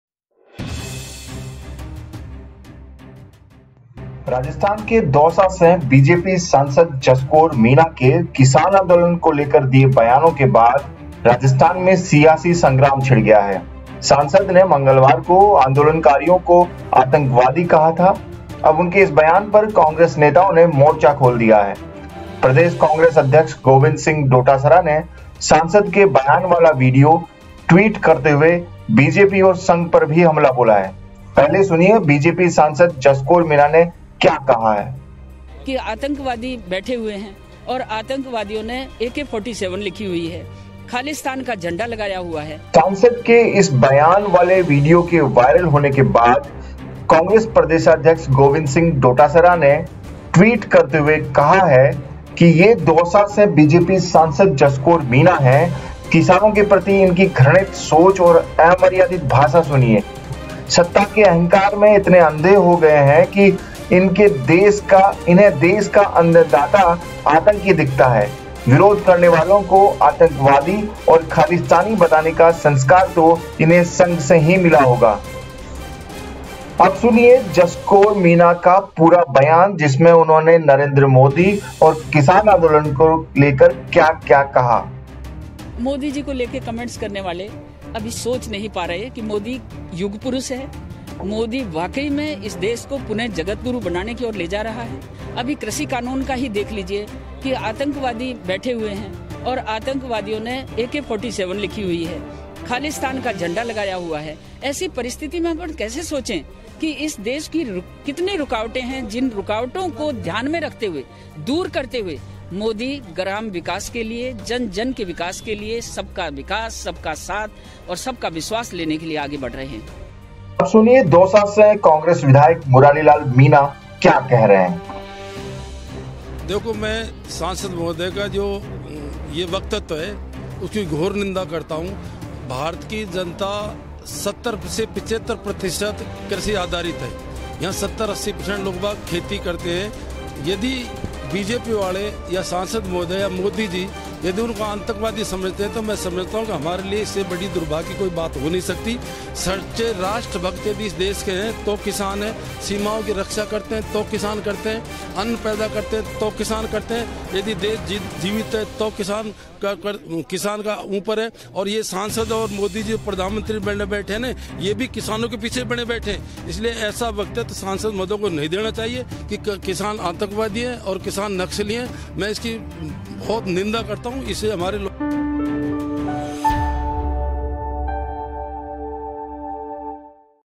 Farmers को Khalistani कहने पर BJP MLA Jaskaur Meena के बयान पर बवाल, सुनें- किसने क्या कहा? | NBT
Farmers को Khalistani कहने पर BJP MLA Jaskaur Meena के बयान पर बवाल, सुनें- किसने क्या कहा?

राजस्थान के दौसा से बीजेपी सांसद जसकौर मीणा के किसान आंदोलन को लेकर दिये बयानों के बाद राजस्थान में सियासी संग्राम छिड़ गया है। सांसद ने मंगलवार को आंदोलनकारियों को आतंकवादी कहा था। अब उनके इस बयान पर कांग्रेस नेताओं ने मोर्चा खोल दिया है। प्रदेश कांग्रेस अध्यक्ष गोविंद सिंह डोटासरा ने सांसद के बयान वाला वीडियो ट्वीट करते हुये बीजेपी और संघ पर भी हमला बोला है।

डोटासरा ने क्या कहा?
पीसीसी चीफ गोविंद सिंह डोटासराने कहा, 'ये दौसा से BJP सांसद जसकौर मीणा हैं, किसानों के प्रति इनकी घृणित सोच और अमर्यादित भाषा सुनिए। सत्ता के अहंकार में इतने अंधे हो गये कि इन्हें देश का अन्नदाता आतंकी दिखता है। विरोध करने वालों को आतंकवादी और खालिस्तानी बताने का संस्कार तो इन्हें संघ से ही मिला होगा।

सांसद जसकौर मीणा ने क्या कहा?
बीजेपी सांसद जसकौर मीणा ने कहा कि प्रधानमंत्री नरेंद्र मोदी देश को जगत गुरु बनाना चाहते हैं लेकिन कृषि बिलों के खिलाफ आतंकवादी सड़कों पर बैठे हुए हैं जिनके हाथों में खालिस्तान के झंडे हैं, उन्होंने AK47 लिखा हुआ है। उन्होंने अपने बयान में आंदोलनकारियों को आतंकवादी तक कह डाला।

दौसा विधायक ने कहा यह अपमान बर्दाश्त नहीं करेंगे
सांसद के इस विवादित बयान पर दौसा विधायक मुरारी लाल मीणा ने पलटवार किया और कहा की भारत के अन्नदाता को आतंकवादी  बताना धरती पुत्र अन्नदाता का अपमान है। किसान आंदोलन की प्रारंभ से देखने में आ रहा है कि बीजेपी नेता आंदोलन को प्रायोजित बताने में तुले हुए हैं। मीणा ने कहा कि बीजेपी के नेता यह सोचते थे कि सीधे-साधे किसान इस बिल को समझ ही नहीं पाएंगे, लेकिन आज का जागरूक किसान इस बिल को समझ गया अब बीजेपी नेता खिसियानी बिल्ली खंबा नोचे वाली कहावत पर उतर आए हैं, यदि यह किसान आतंकवादी हैं तो भारत की 80% जनसंख्या खेती पर आधारित है देश की रक्षा करने वाले बॉर्डर पर  सैनिक  धरतीपुत्र है, तो देश का 80% लोग कैसे आतंकवादी हो सकता है। उन्होंने सांसद के किसान विरोधी बयान की पूर्ण निंदा की और इसे धरती माता का अपमान बताया।